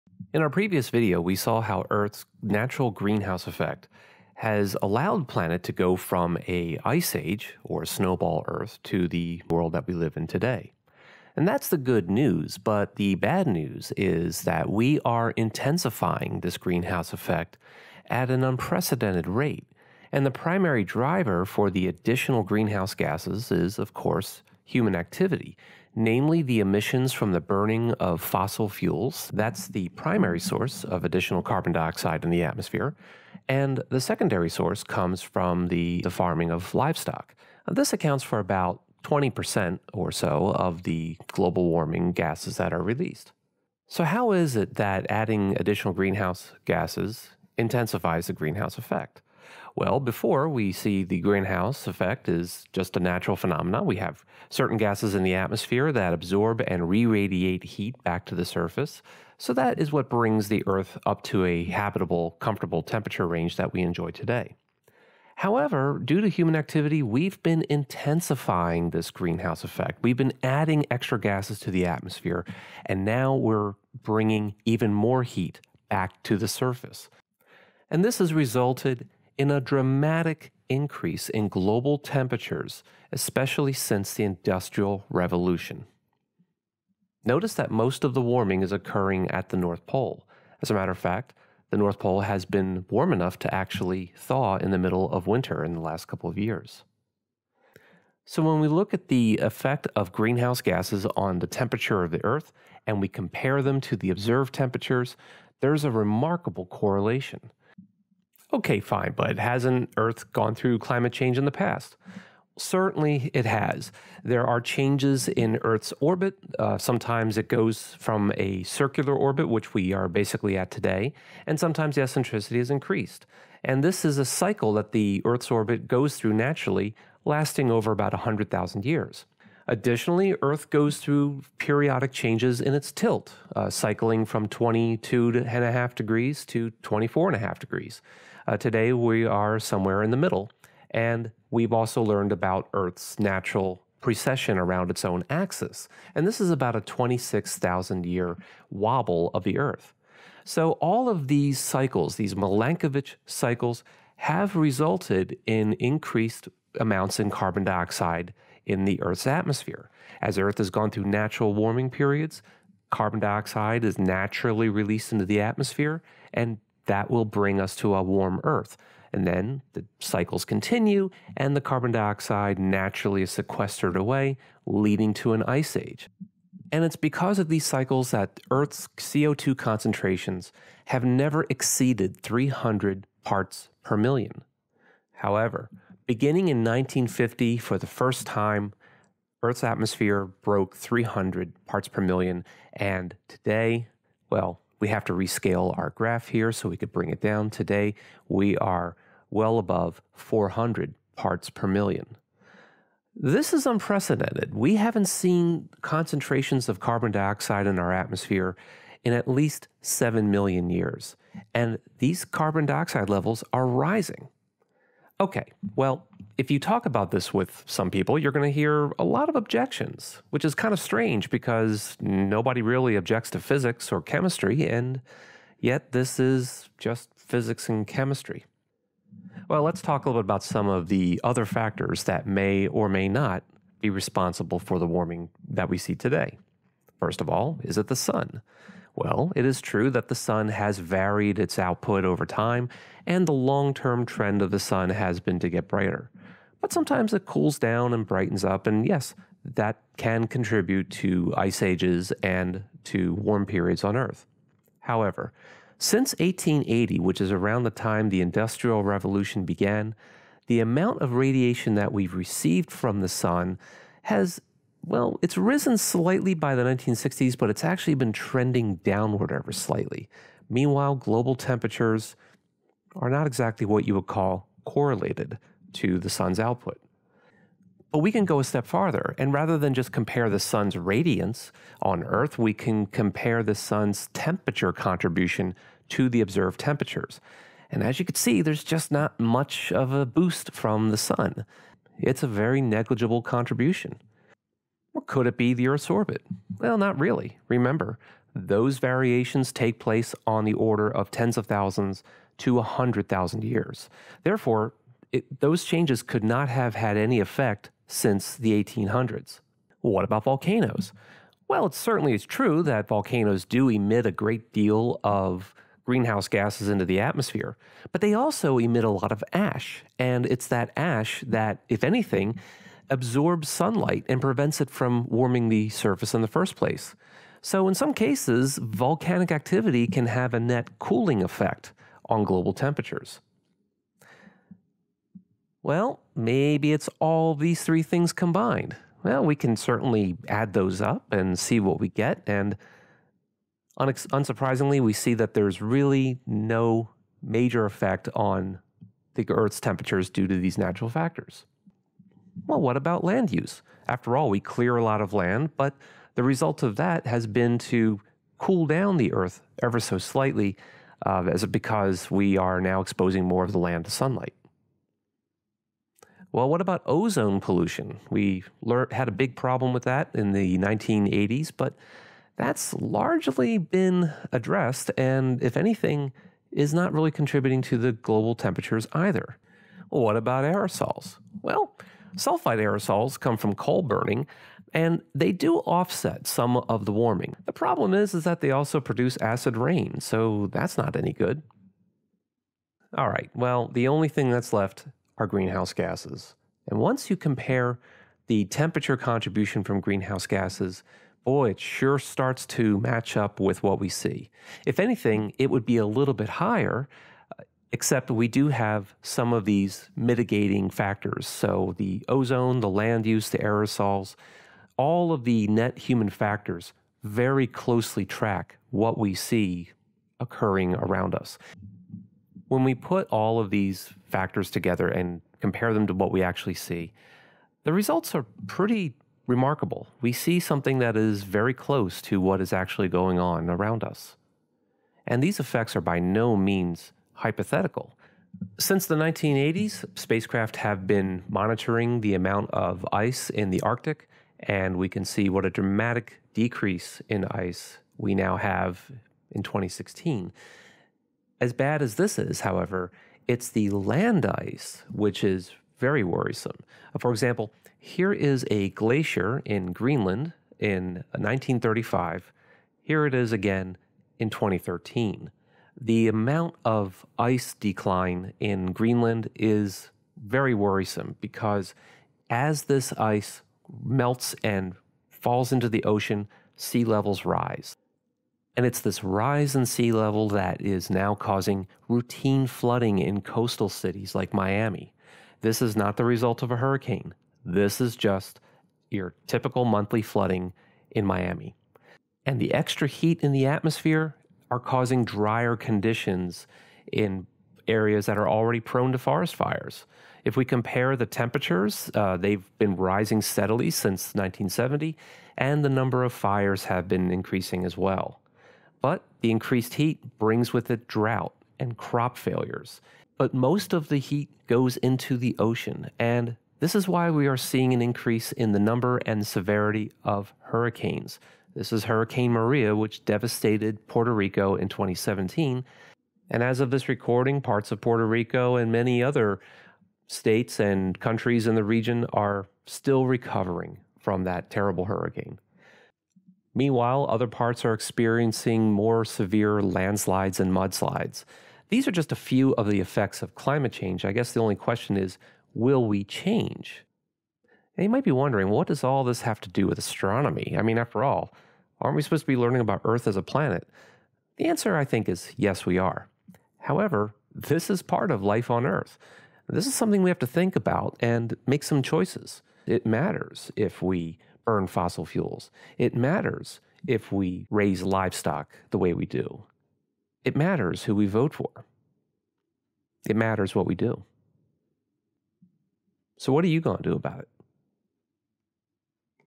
How Humans Cause Climate Change
A look at how humans cause climate change / global warming by adding increased greenhouse gasses to the planet's atmosphere, and how this extra heat trapping ability manifests itself in rapid climate change.